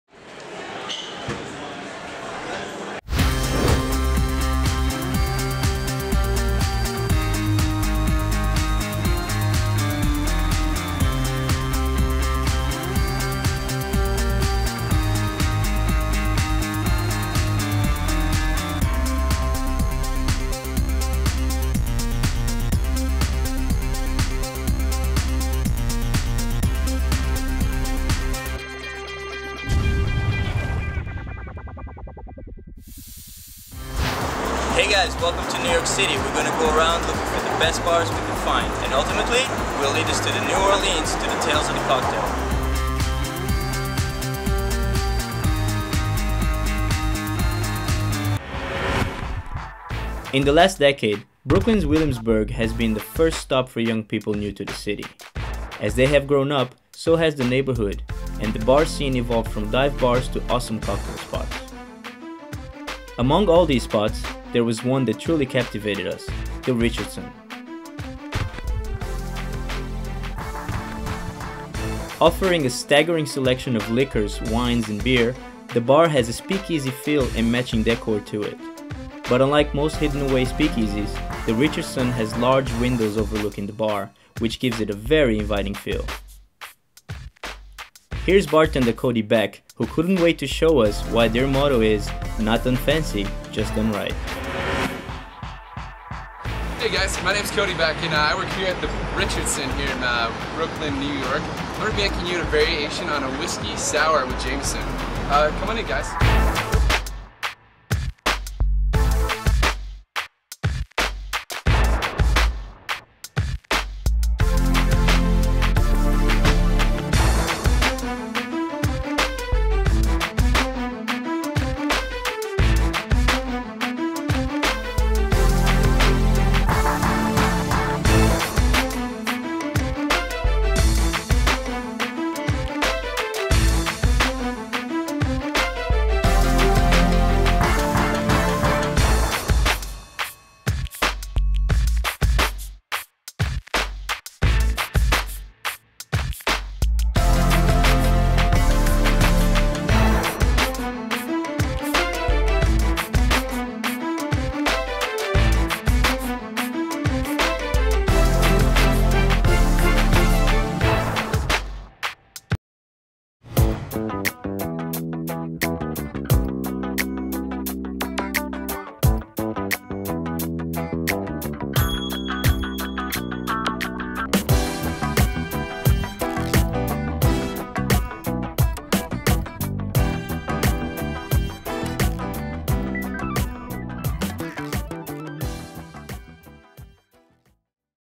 The Richardson - Brooklyn, N.Y - Episode 2.
We are on a journey through the U.S to find 9 of the top bartenders with their best recipes of their own created Cocktails.

Filmed in 3 major cities in the U.S
NEW YORK - MIAMI - NEW ORLEANS.